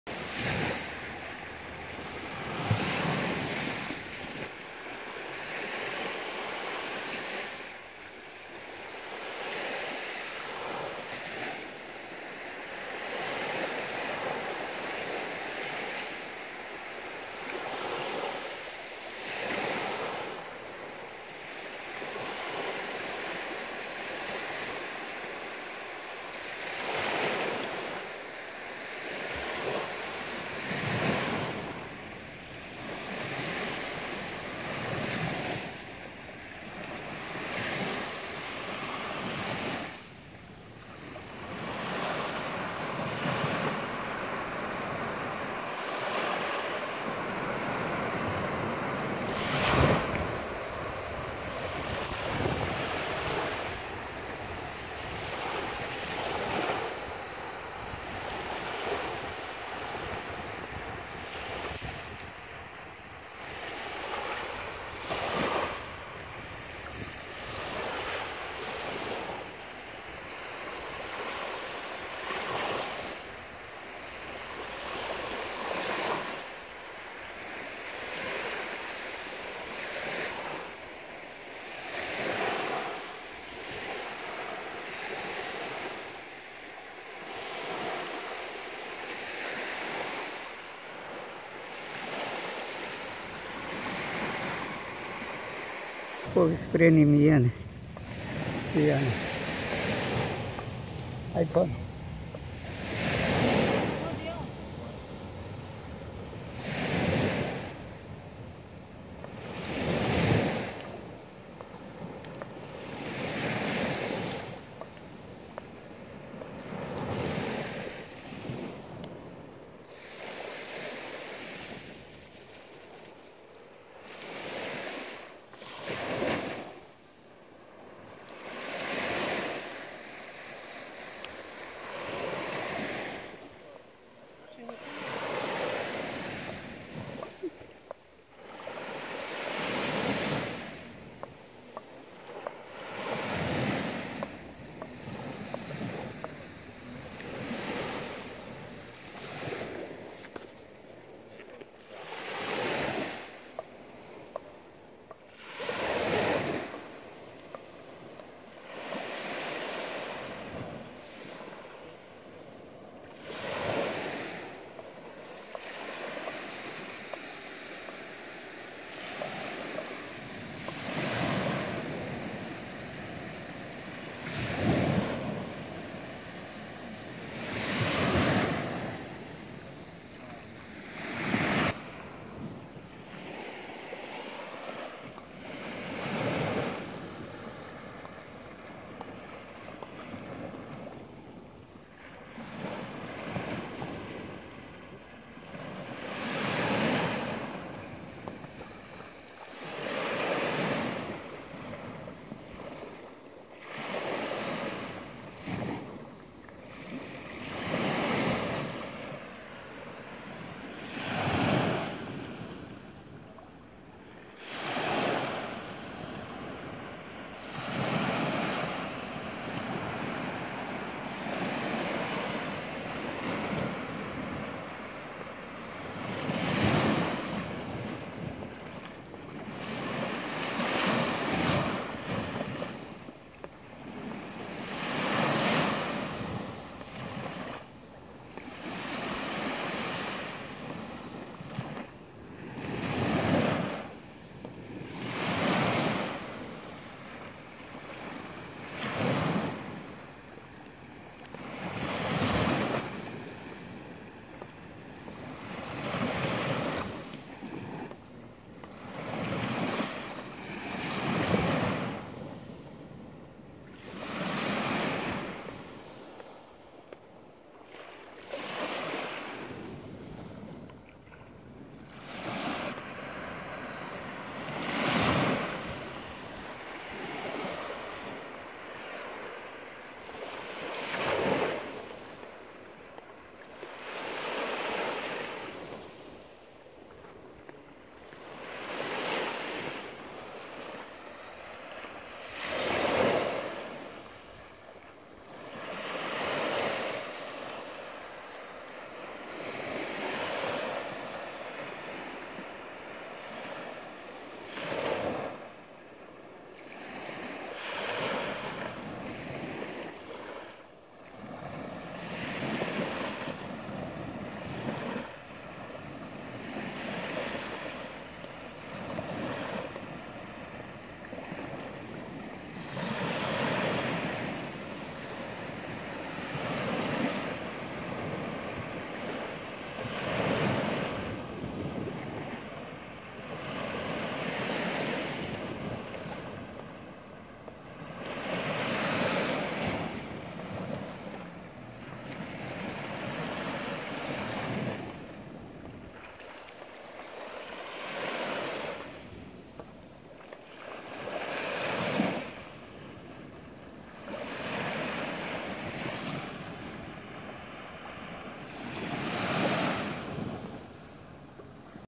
【4K】 Greece Zakynthos Beach - Paralia Gerakas Beach with Sea Waves Sounds
4K Beach View in Greece - Gerakas Beach | Gerakas Beach, Zakynthos | Παραλία Γέρακας
Plaża Gerakas, Grecja | 그리스 자킨토스 게라카스 해수욕장
Lovely beach, nice sand, dotted with beach bars and loungers |
The water is very shallow for a long way out...

Gerakas is the southern beach of the island and one of the most beautiful.
It consists of a long sand tongue ending in a small promontory diving in the sea.
This beach is a protected area of the Zakynthos Marine Park because of the sea turtles’ choice to go there to hatch.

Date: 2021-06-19
Time: 10h55